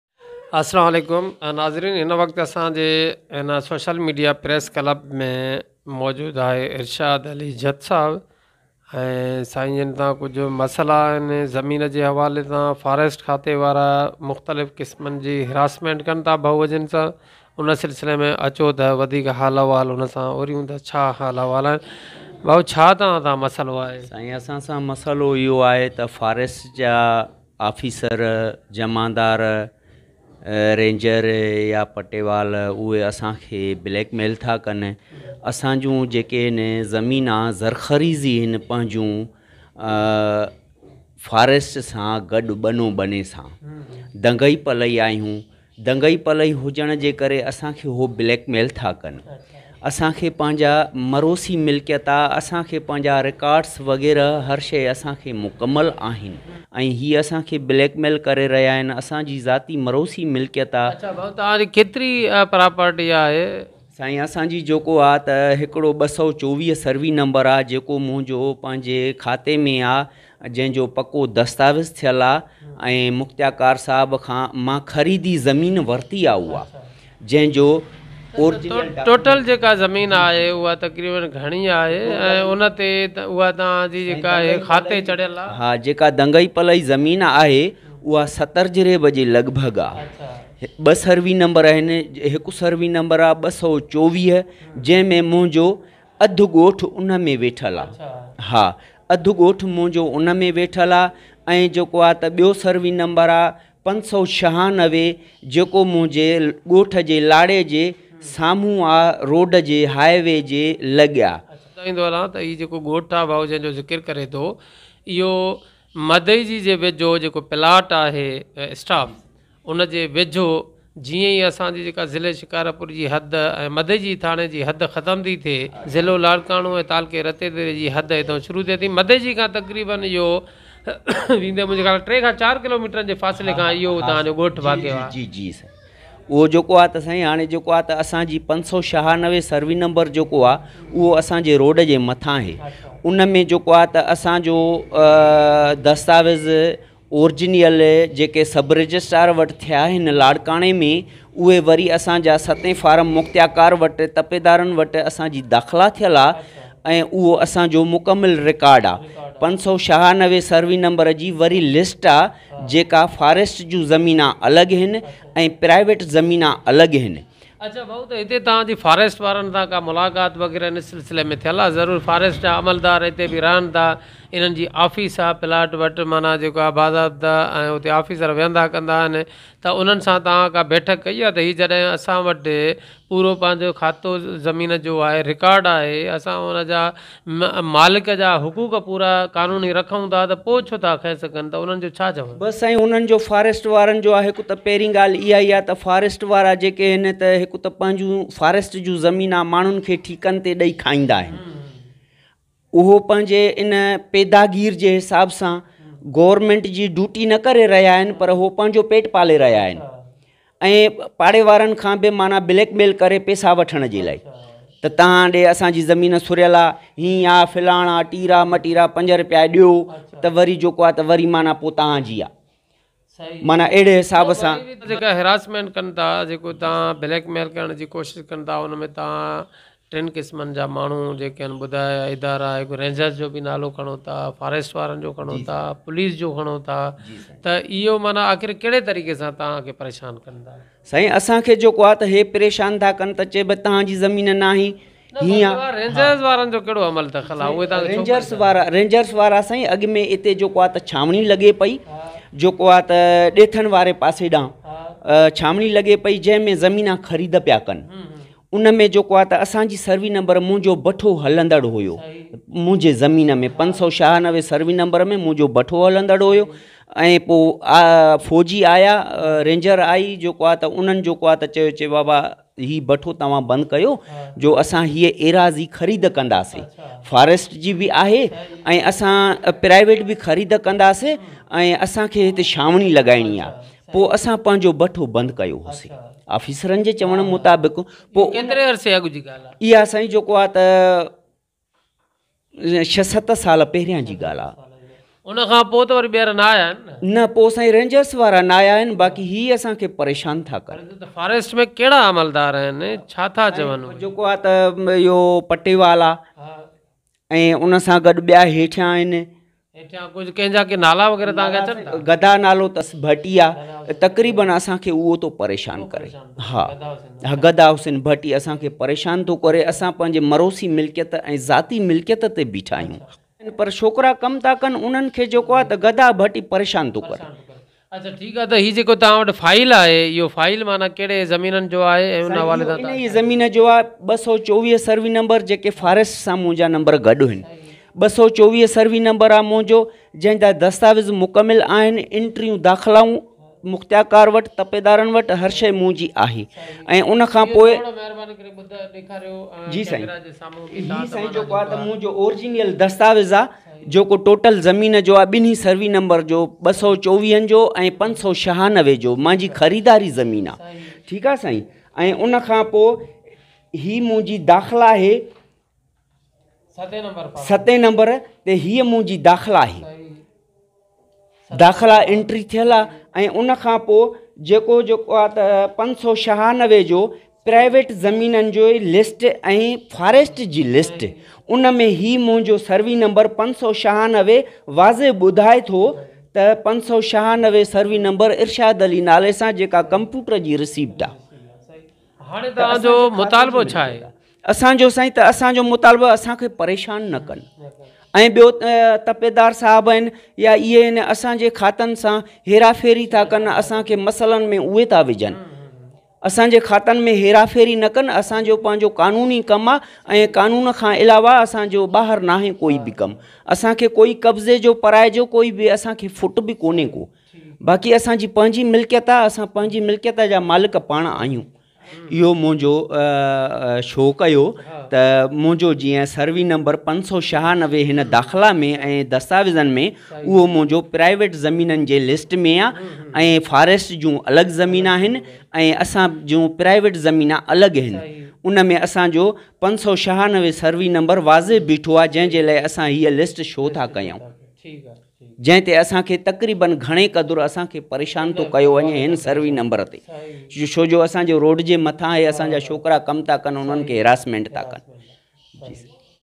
forest amaldaron ky khilaf irshad jat ka ihtjaj
مدئجي/فاريسٽ عملدار اسان کي بليڪ ميل ٿا ڪن,مان فاريسٽ وارن سان دنگئي پلئي آهيان,فاريسٽ وارا سرڪاري زمين جي حفاظت بجاء پنهنجي پيٽ پوڄا جي ڪڍ آهن,سماجي اڳواڻ ارشاد علي جت جي احتجاجي پريس ڪانفرنس